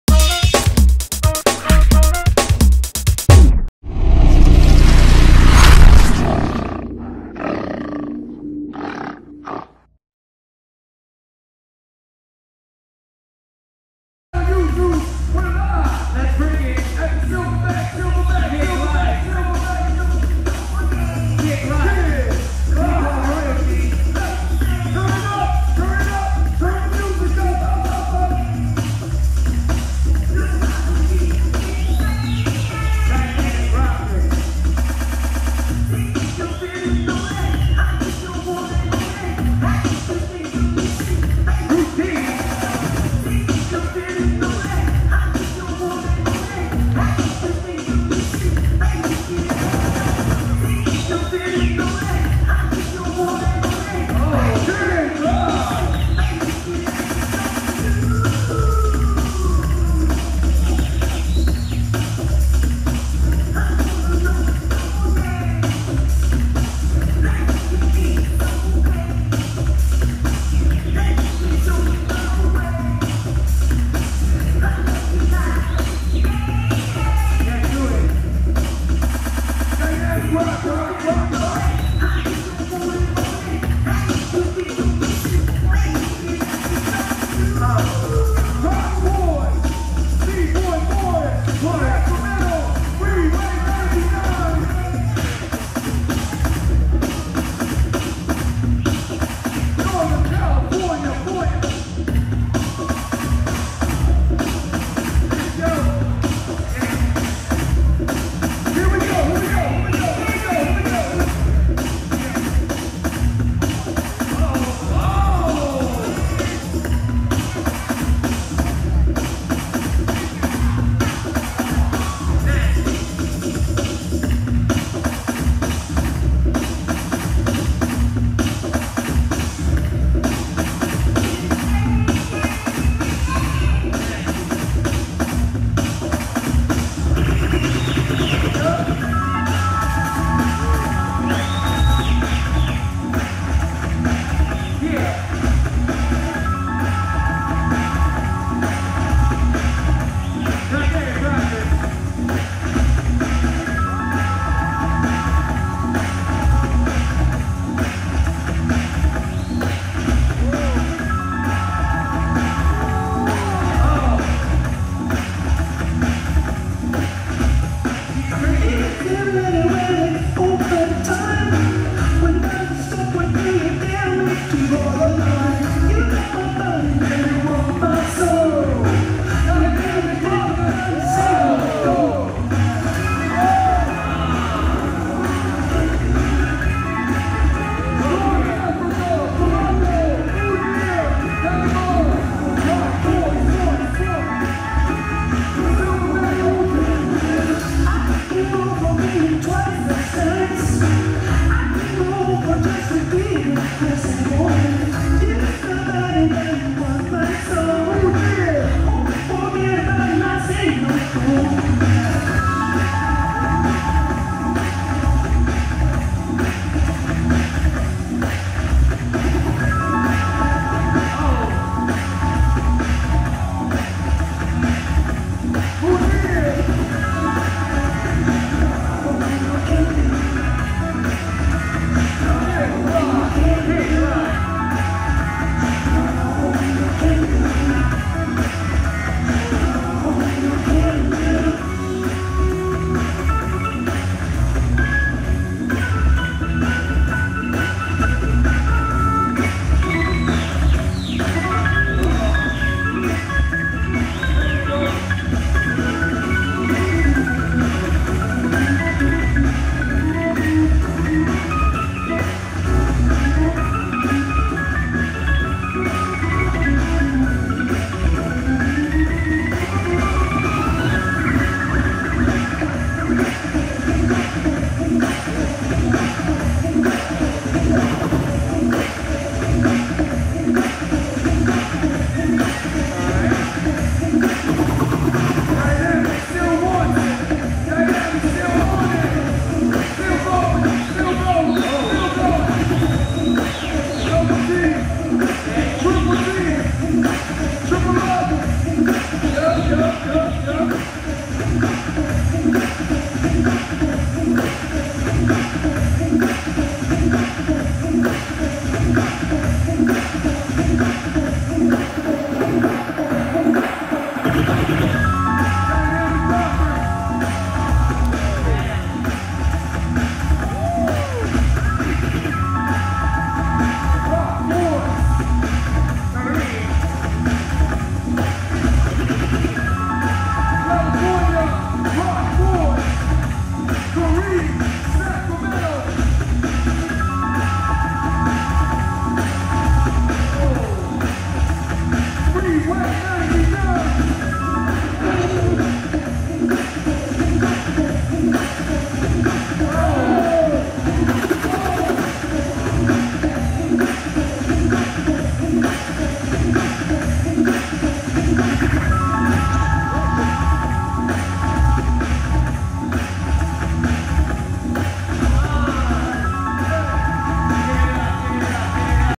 Rock Force vs Dynamic Rockers | Silverback Open 2015 | UDEFtour.org x Strife | Top 32
Winer:

Rock Force Crew
Kareem, Morris, Mounir